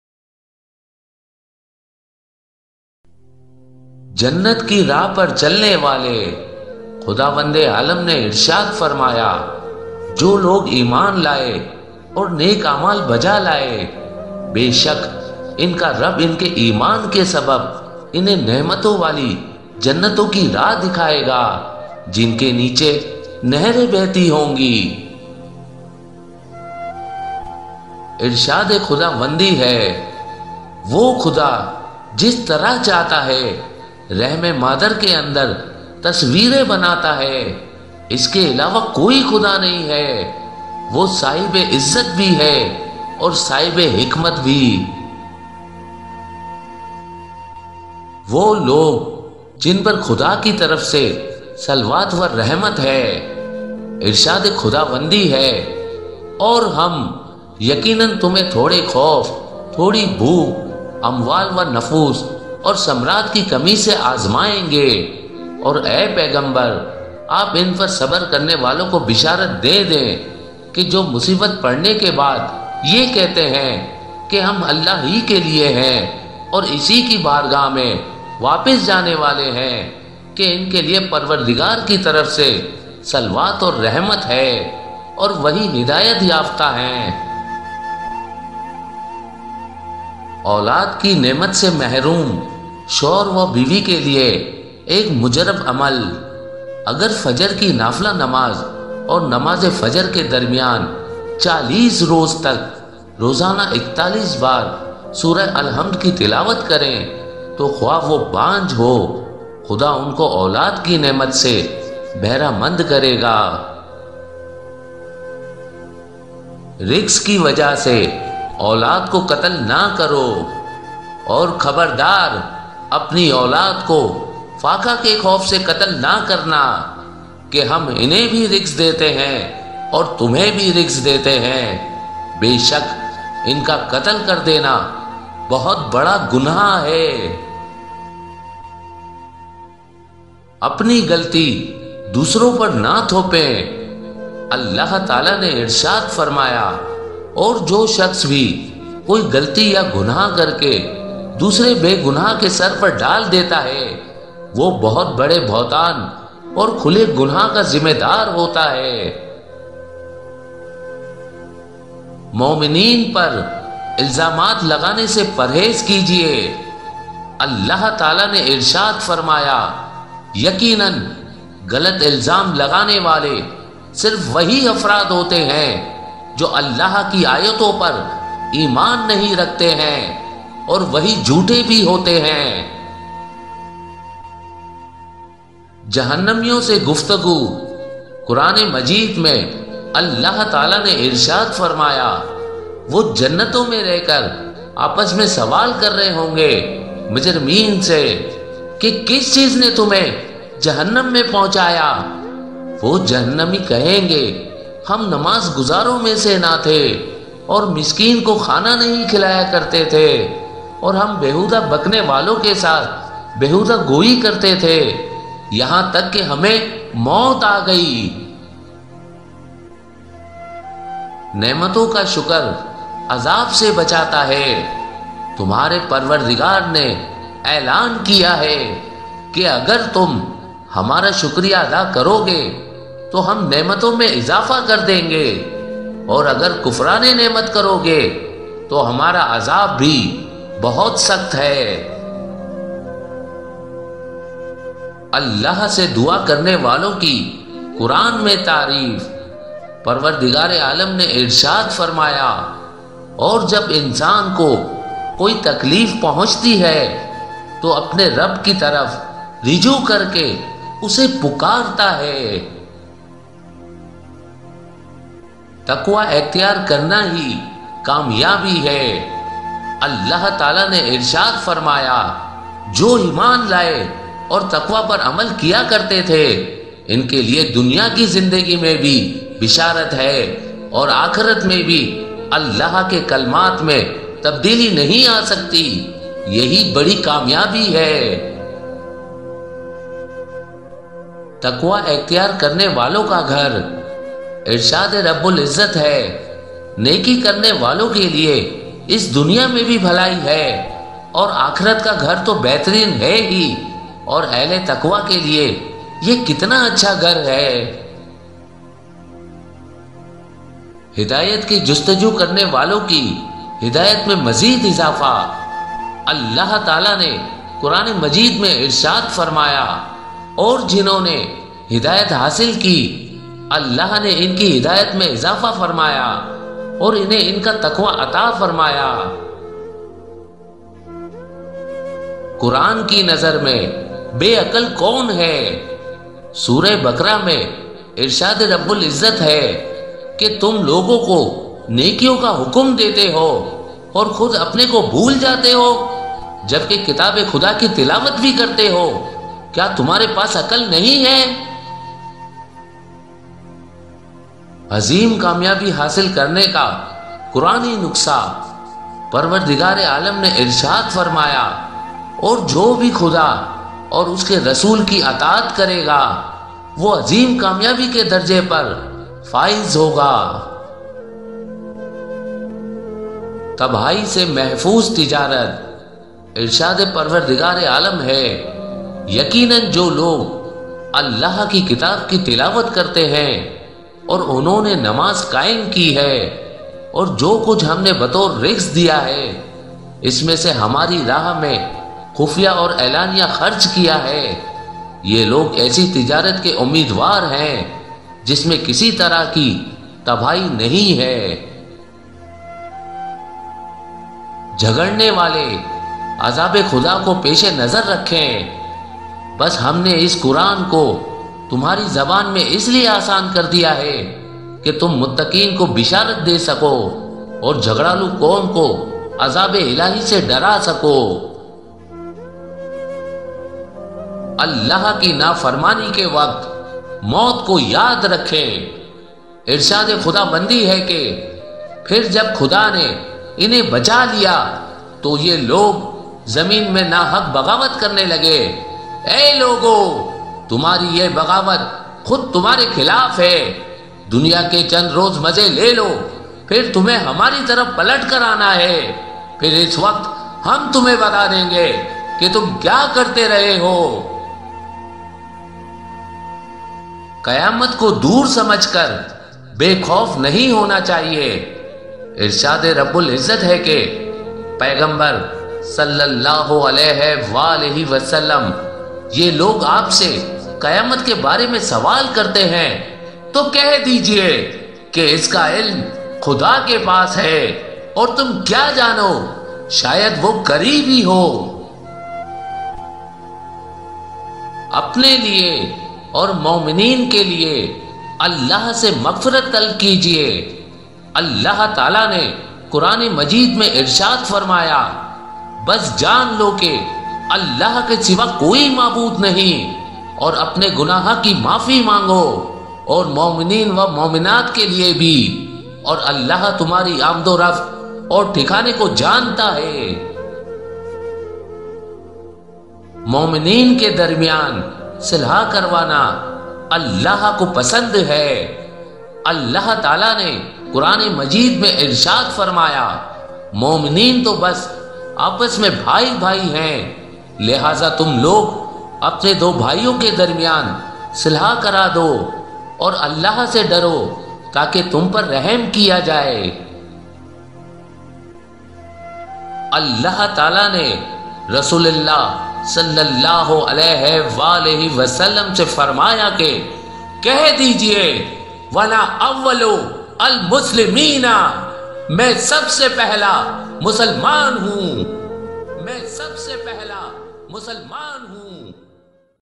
Allah Ki Nafarmani Karne Wale | Maut Ko Yaad | Quran Ka Farman | Shia | Allah Mehrban | Merchant Tv
Allah Mehrban Quran Me Farmata Hai Allah Ki Nafarmani Karne Wale Maut Ko Yaad Rakhna